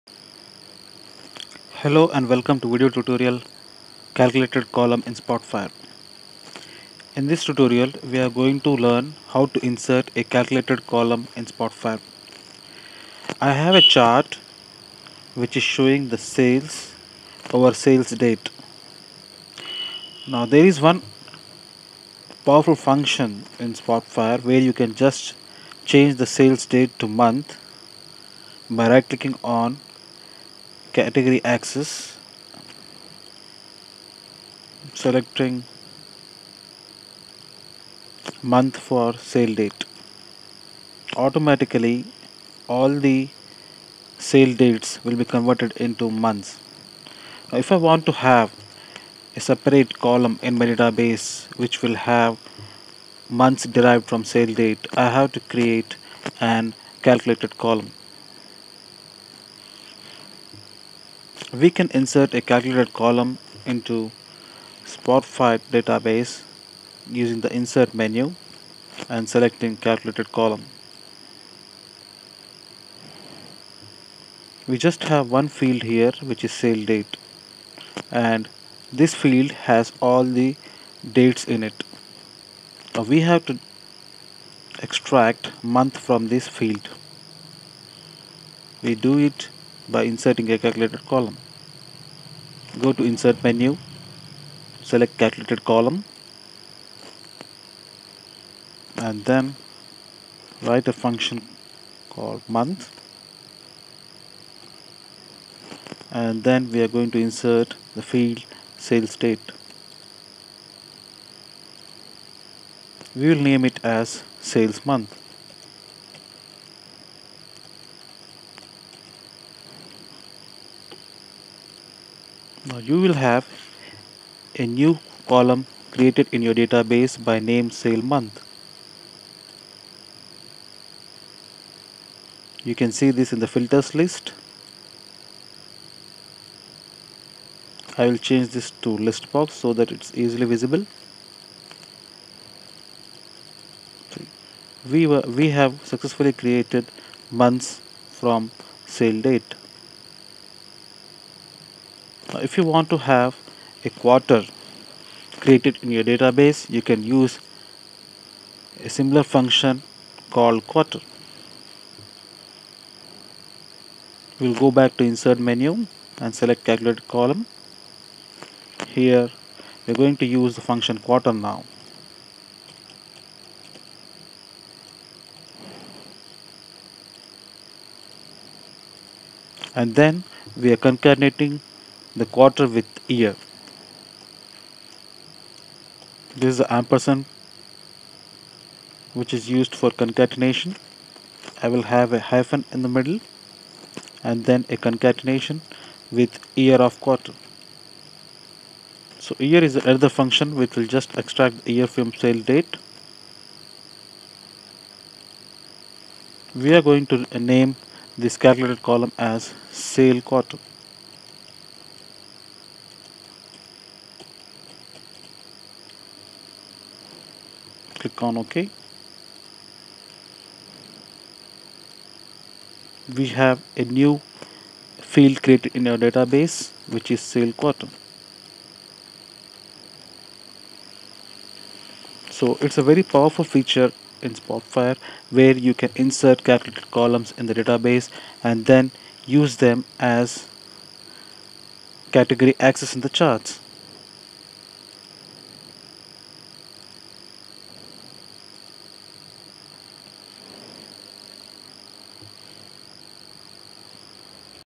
TIBCO Spotfire : Calculated Column
This is a video tutorial about including calculated column in TIBCO Spotfire